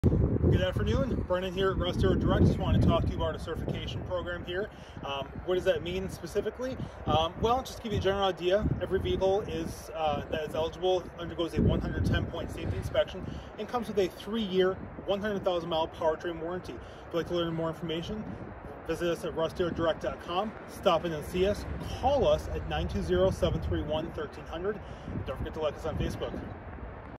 What is the Darrow Certification Program? - Russ Darrow Direct
Looking for a brand new vehicle, but don't want to break the bank? A Darrow Certified vehicle is the way to go! Other dealerships may promise that their vehicles are solid, in great shape and without problems, but our dealership warrants the condition of these vehicles with a 3 year/100,000 mile Certified Protection Warranty.